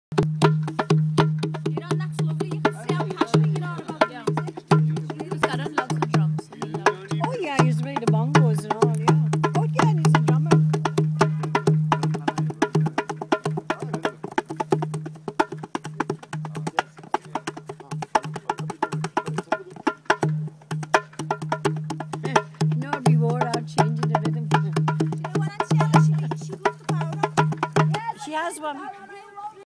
Dholak around a fire in the Himalayan Mountains
Kiran Gandhi
Dholak around the fire in Dehradun, India
Christmas 2011